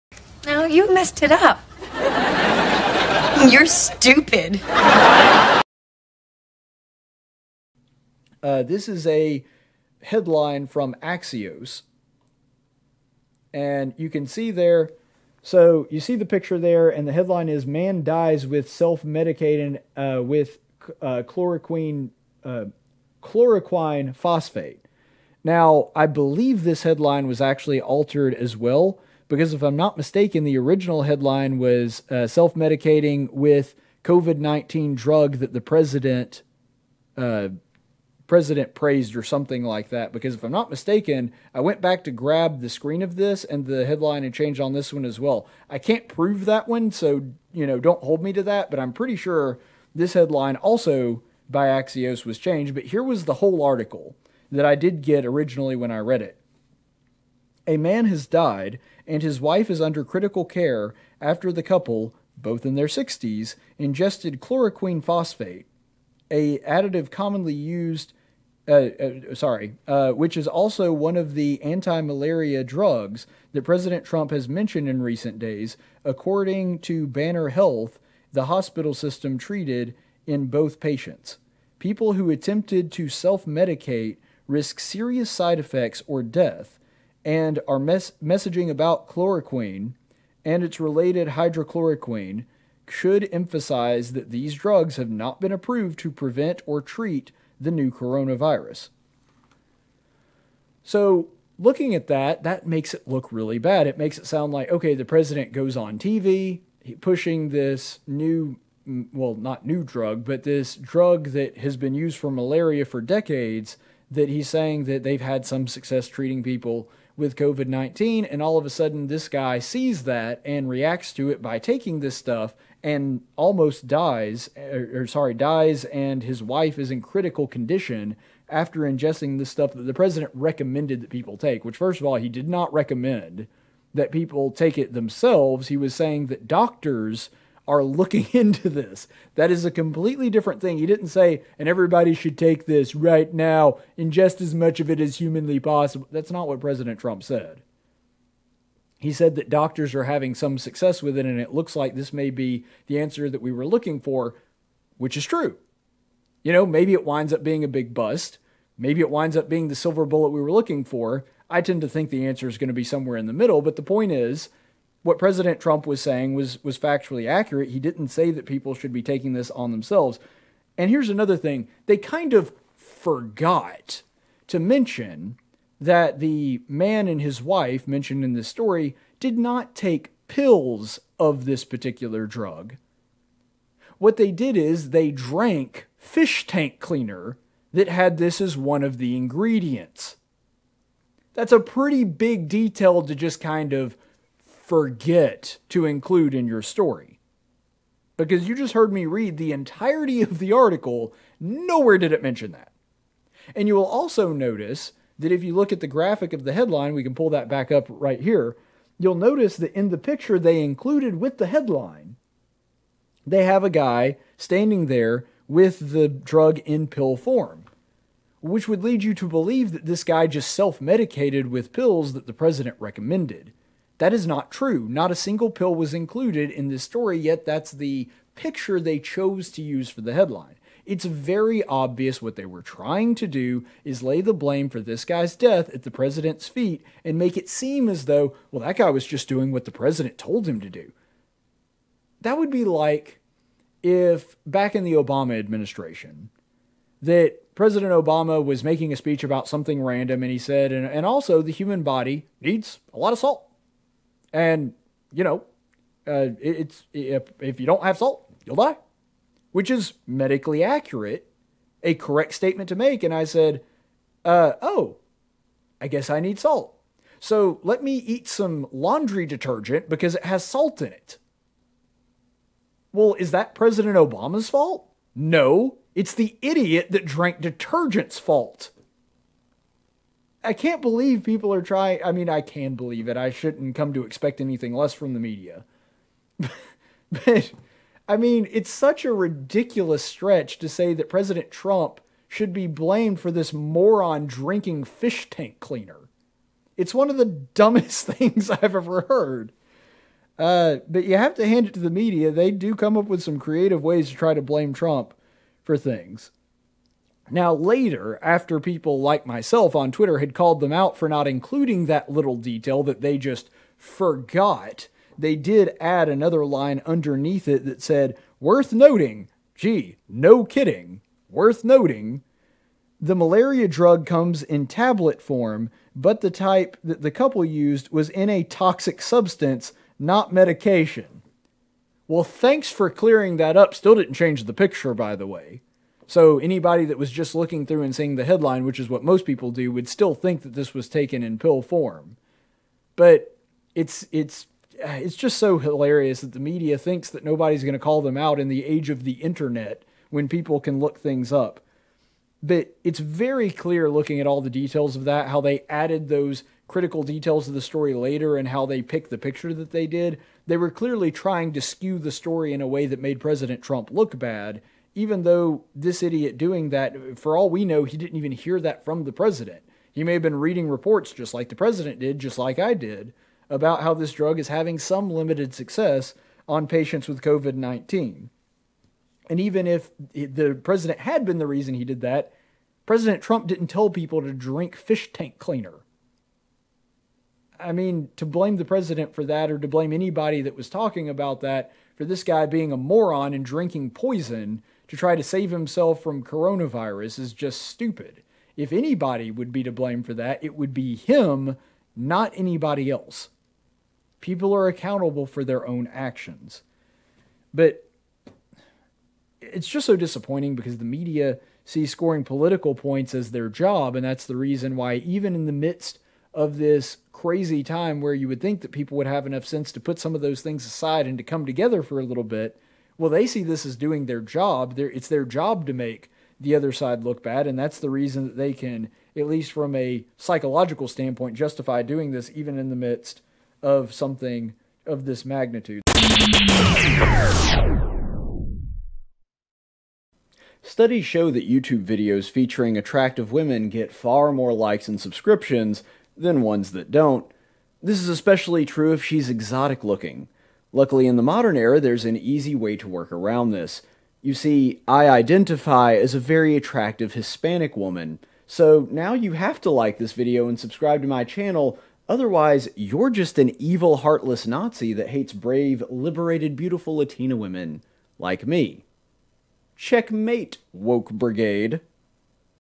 DDoS- Media Blames Trump For Guy Drinking Fish Tank Cleaner
President Trump talks up a drug that might help fight the Kung Flu, the media blames him when a guy drinks industrial cleaner with the same ingredient.